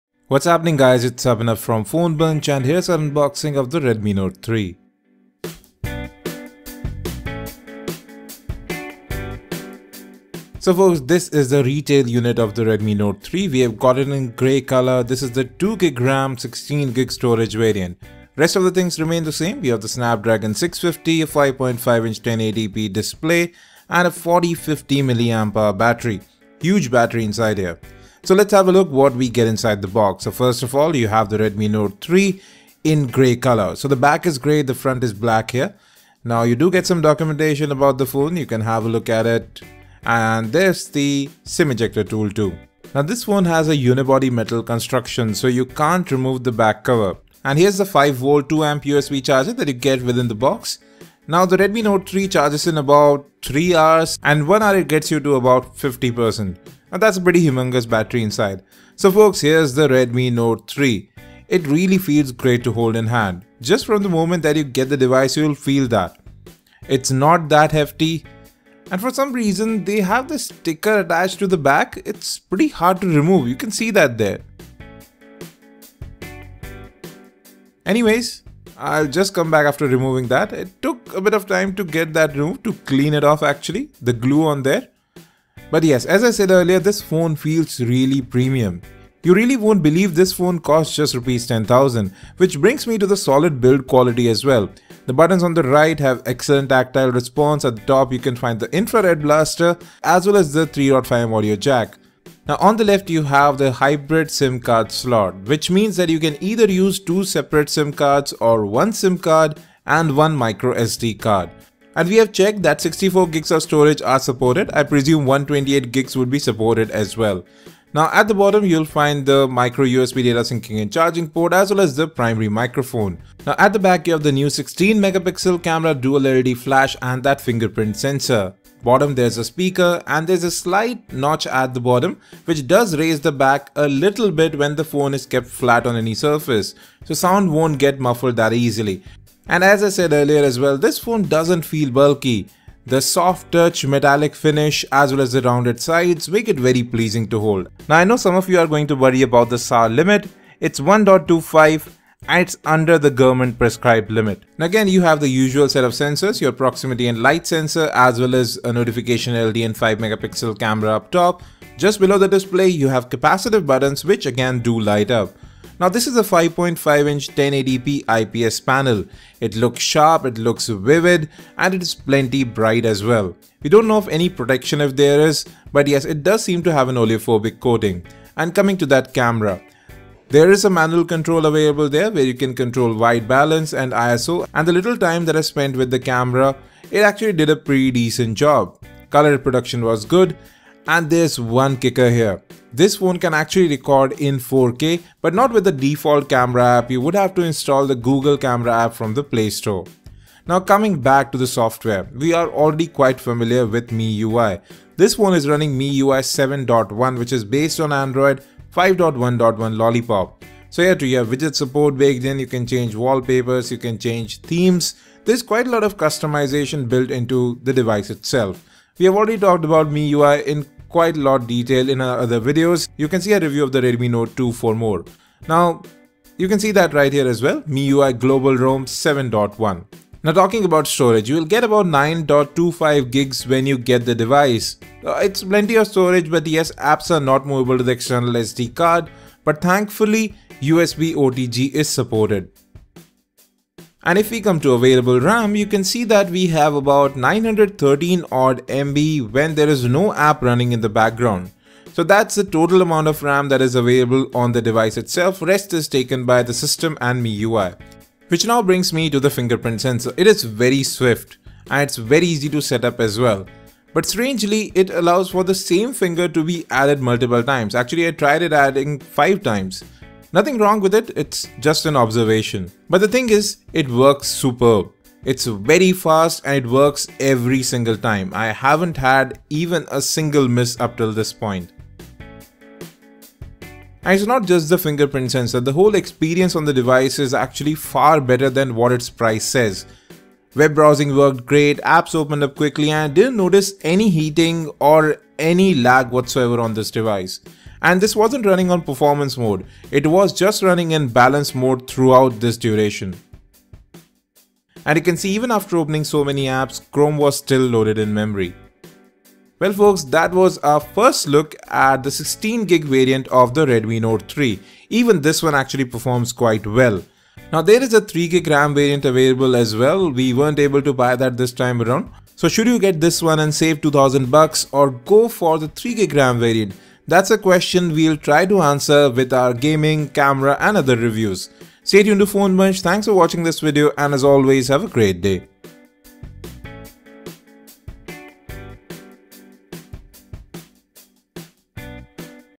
Xiaomi Redmi Note 3 (2GB RAM) India Unboxing
Xiaomi Redmi Note 3 (2GB RAM) unboxing with look at camera, USB OTG support, available RAM, storage. Redmi Note 3 was launched in India with Snapdragon 650, 2GB/3GB variants with 16GB/32GB storage running Android 5.1.1 Lollipop based MIUI 7.1.